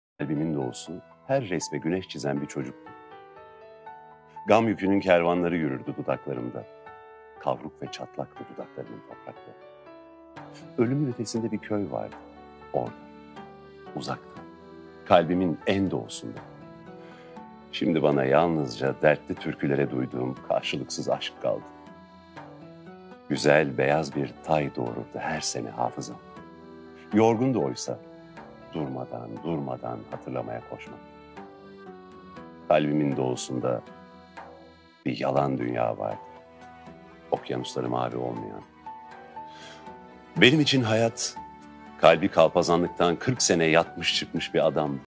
Kalbimin En Doğusunda - Didem Madak (seslendiren: Yetkin Dikinciler)
Kalbimin En Doğusunda
Aşkın kanununu tahsil etmiştim kalbimin en doğusunda
İçimde yağmur duasına çıkmış birkaç köy
Birkaç köy sular altında.
Kalbimin doğusu,
Her resme güneş çizen bir çocuktu.
Gam yükünün kervanları yürürdü dudaklarımda
Kavruk ve çatlaktı dudaklarımın toprakları.
Ölümün ötesinde bir köy vardı
Orda, uzakta, kalbimin en doğusunda
Şimdi bana yalnızca
Dertli türkülere duyduğum karşılıksız aşk kaldı.

Güzel beyaz bir tay doğururdu her sene hafızam
Yorgundu oysa
Durmadan, durmadan hatırlamaya koşmaktan.

Kalbimin doğusunda bir yalan dünya vardı.
Okyanusları mavi olmayan.
Benim için hayat,
Kalbi kalpazanlıktan kırk sene yatmış çıkmış bir adamdı.
Geçmişim acıyor şimdi, yalnız benim değil
Benim ülkemin geçmişi de acıyor mesela.
Bilirdim oysa ilk badem ağaçları çiçek açar baharda.
Bilirdim çiçek satan çingene kızlarını
Onlar bütün şimdileri, bütün zamanlara
Bir gül parasına satardı.
Oğlan kıza bir gül alsa
Bilirdim odur en kırmızı zaman.
Adına aşk diyorlardı
Kalbimin en doğusunda bir yalan dünya vardı.

Kim bir şairi kırsa
Şair gider uzun bir dizeyi kırar mesela
Bilirim kim dokunsa şiire
Eline bir kıymık saplanacak.
Bilirim kırılmış dizeleri tamir etmez zaman
Yorgunum oysa
Durmadan kendime bir tunç uyak aramaktan.

Boş salıncaklar gibi gıcırdayarak konuştum karanlıkla
Kediler gibi mırıldanarak.
Alkolden bir denize bıraktım kalbimi
Kırmızı bir sandal gibi,
Arka sokaklarda sarhoş konuştum karanlıkla.
Avuçlarımla konuştum,
Allah büyüktür diyen insanlar gibi.
Kedi dili bisküvilerinin bir pastayla konuşması gibi
Yumuşak ve kremalı konuştum onunla.
Baharda leylaklar açardı boynumda
Mor ve pembe konuştum karanlıkla
Gece açılıp gündüz kapanan bir parantezdim,
Sözler vardı içimde işe yaramayan
Sözlerle konuştum karanlıkla...
Önce söz yoktu kalbimin en doğusunda
Sözler...
Bir yağlı urgandı acıyı boğmaya yarayan.